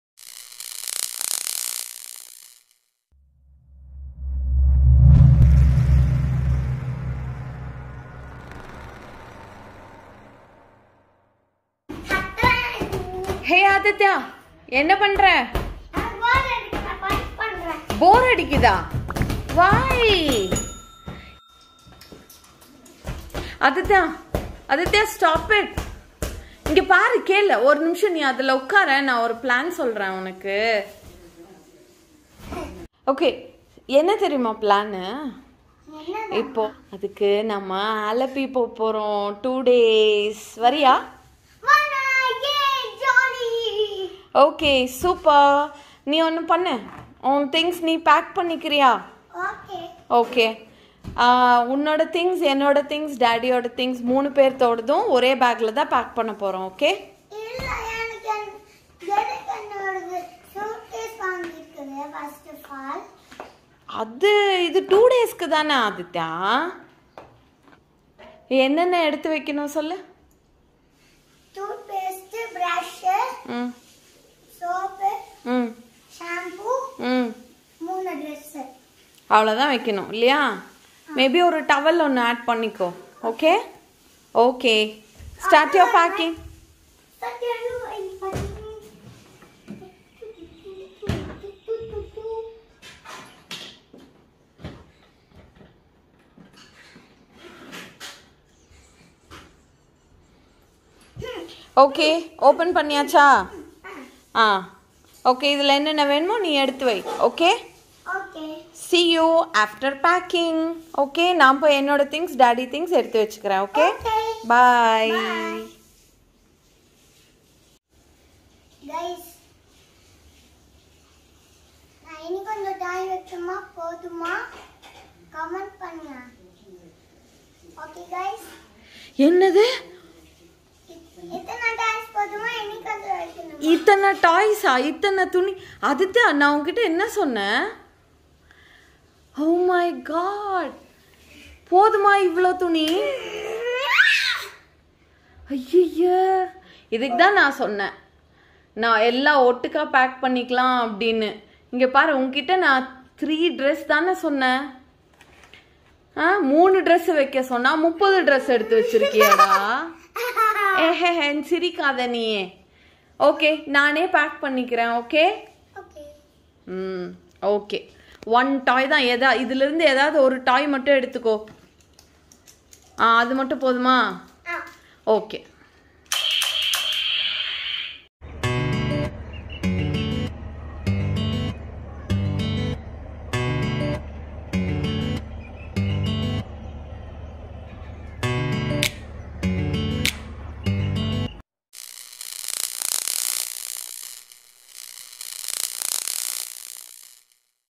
Packing things for a trip by kids | Fun packing by kids | A trip to Allepy
It's always fun to watch kids pack their luggage for a trip.